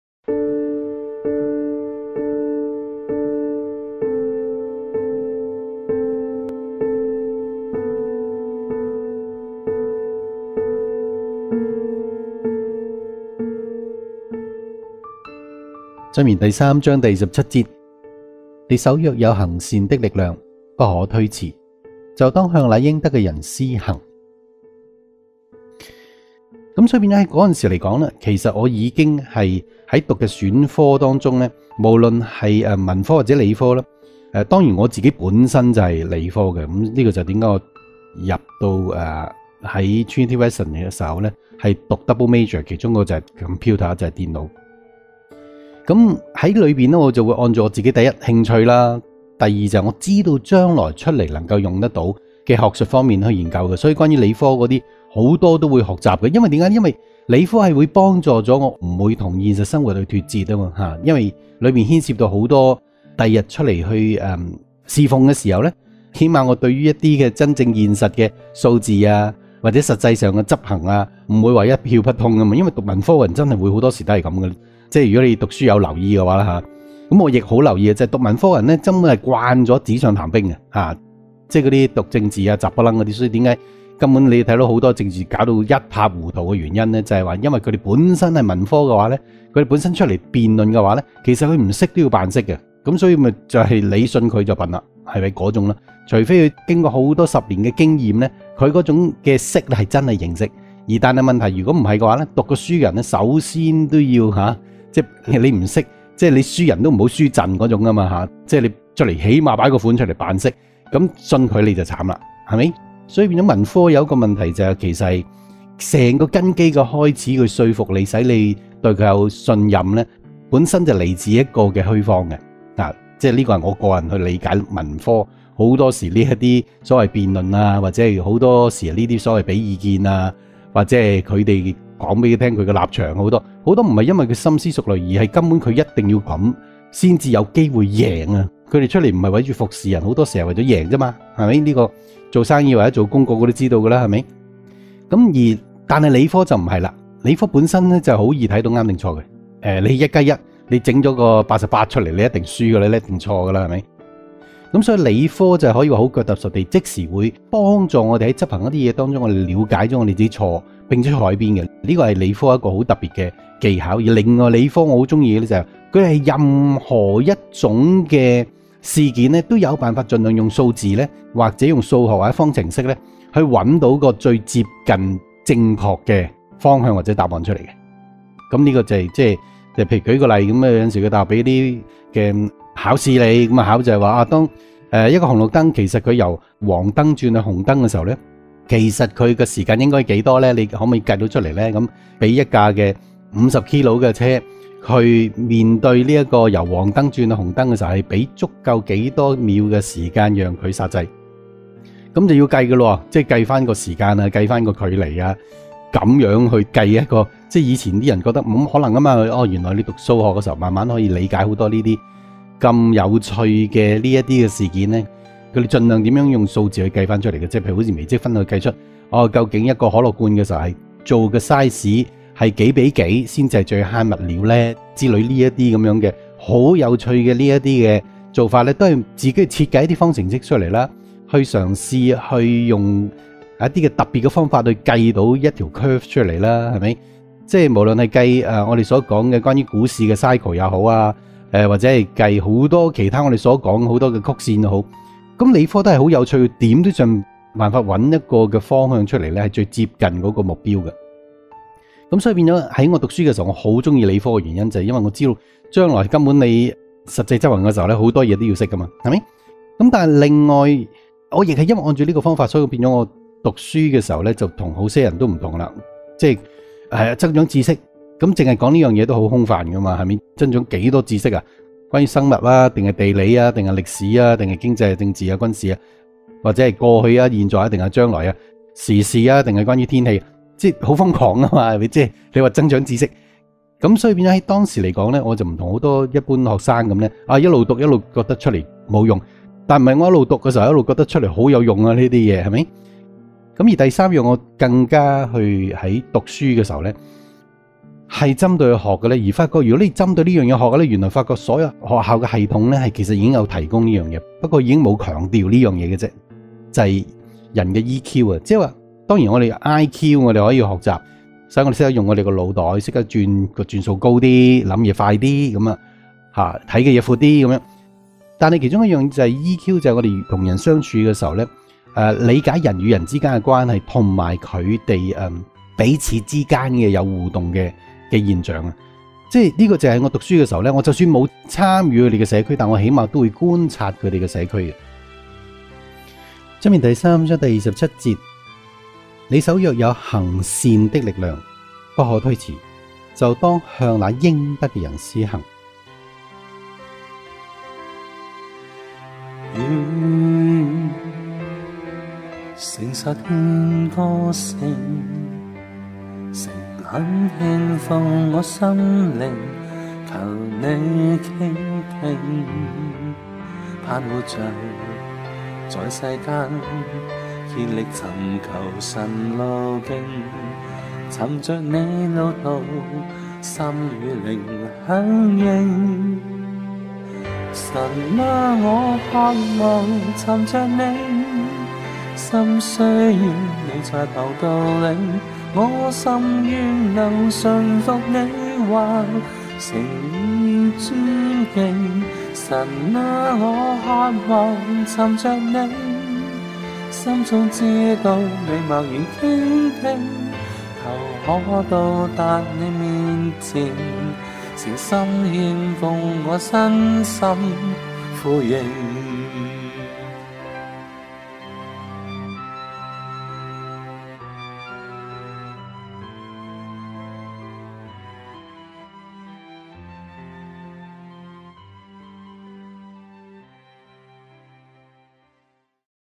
錫安教會靈修系列：栽在溪水旁 190419
箴言 3章27節：
「你手若有行善的力量，不可推辭，就當向那應得的人施行。」